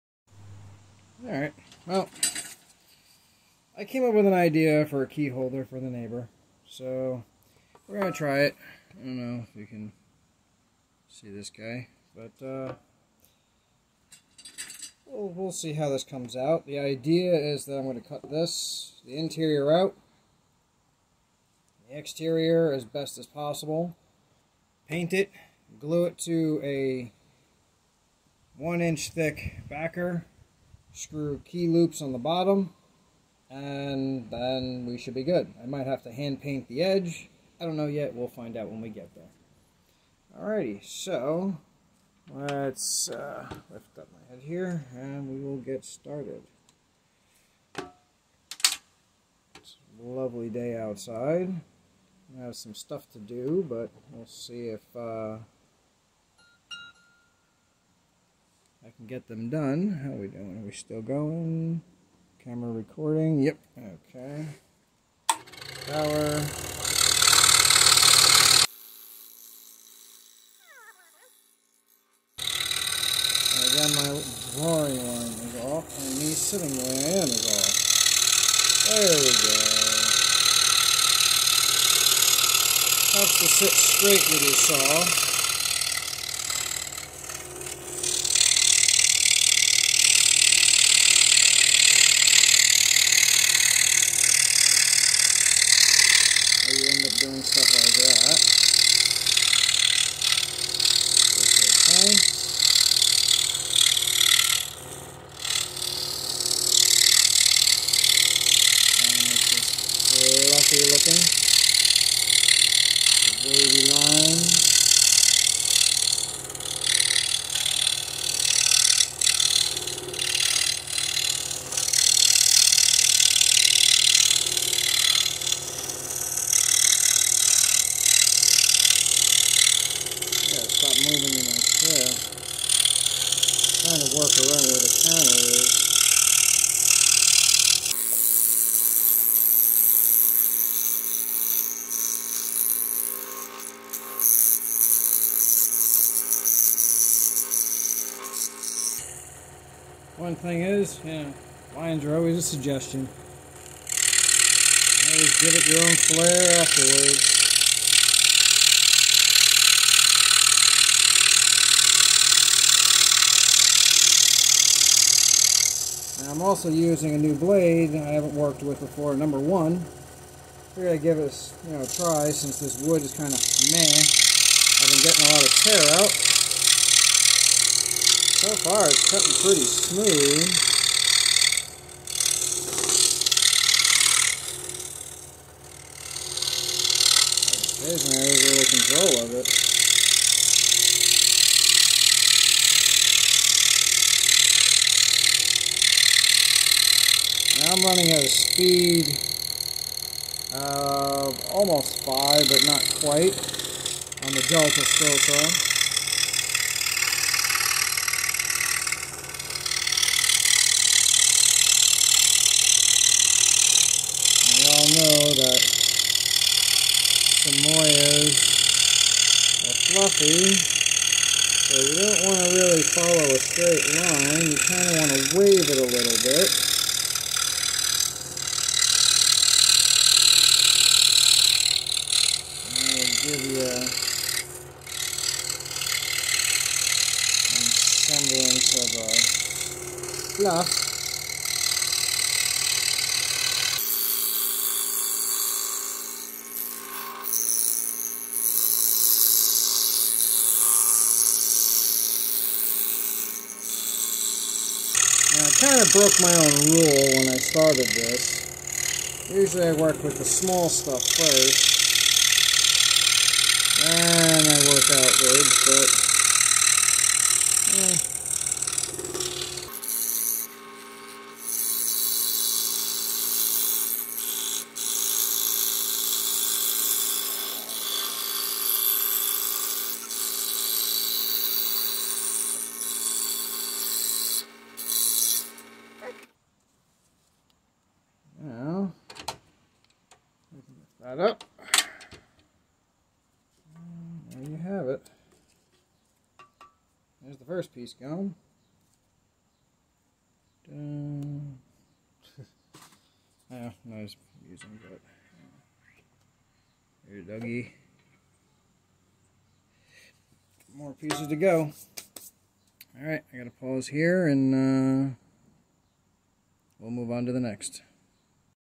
Samoyed Wall plaque Keys Holder Part 1
1st in a mini series about the Samoyed plaque keychain holder. Not sure how many this will be. Plans to show start to finish from cuts to paint to Keyhole hangers router bits.